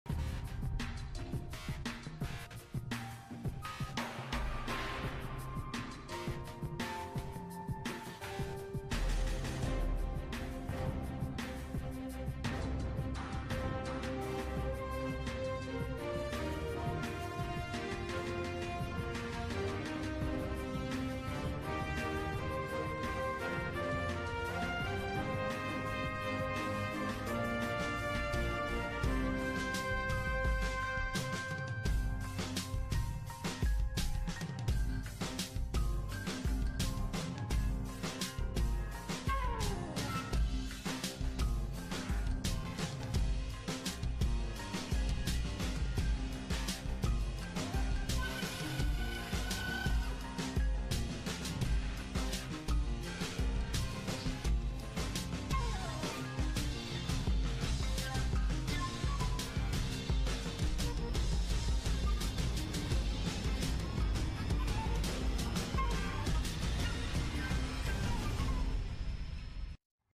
Gonzalo de la Carrera sobre el Estadio Nacional: "Deberían dejarlo así"
Diputado.